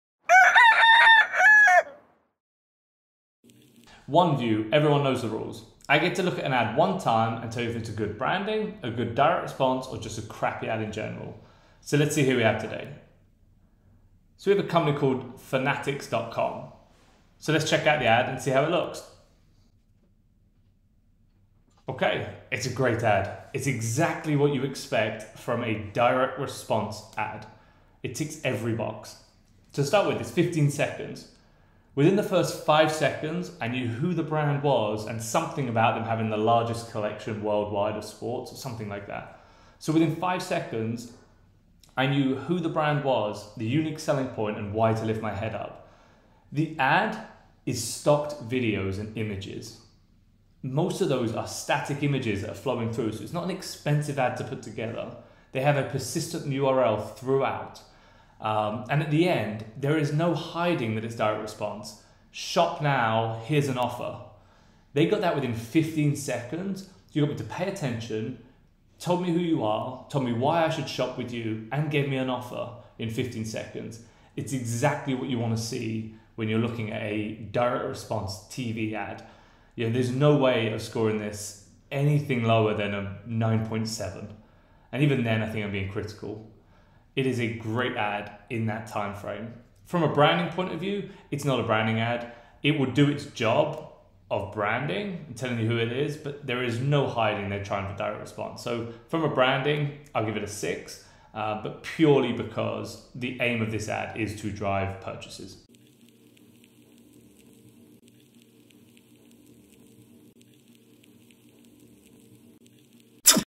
One View TV Commercial - Fanatics.com
One View by Keynes Digital

Everyone knows the rules. Dan Larkman gets a chance to look at a TV ad one time and tell you if it's a good Branding, a good Direct Response ad, or just a crappy ad in general. Dan is specifically looking at Connected TV.